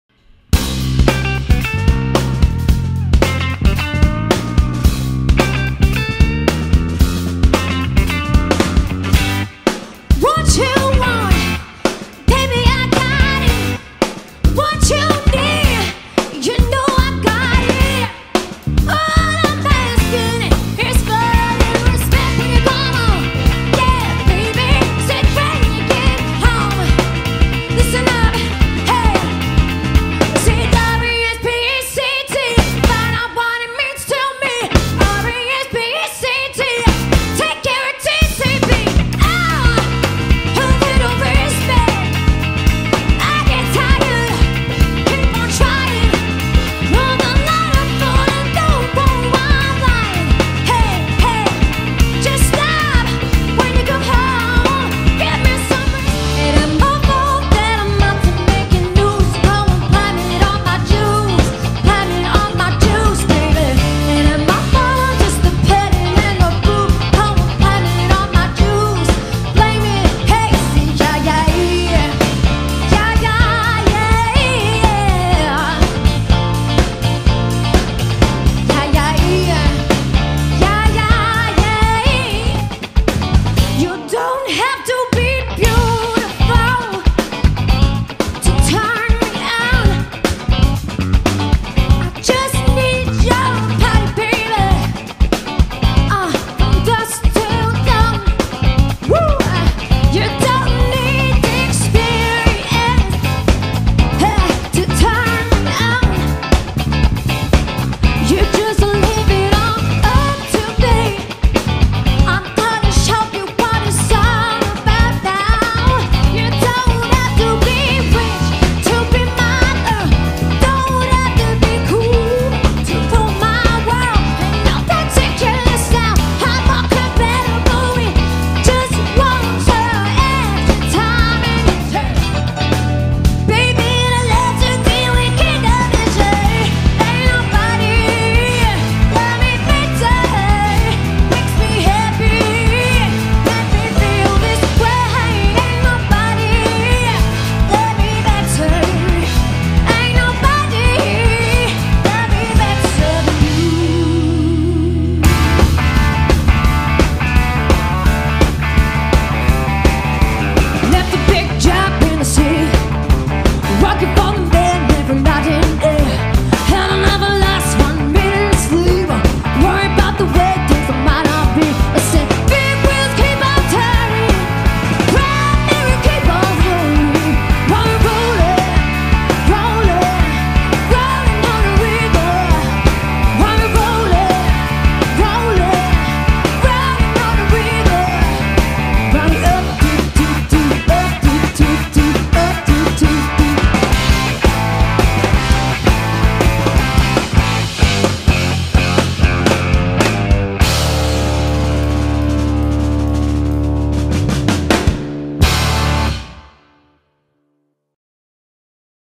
Sugar Rush - Ultimate Show Reel - AliveNetwork.com
Sugar Rush, The band that gives your event the soul and magic it deserves. Give your guests a night they won't forget with powerful vocals and a huge energy performance. Tunes from Aretha Franklin, James Brown with some surprises for rock fans as well!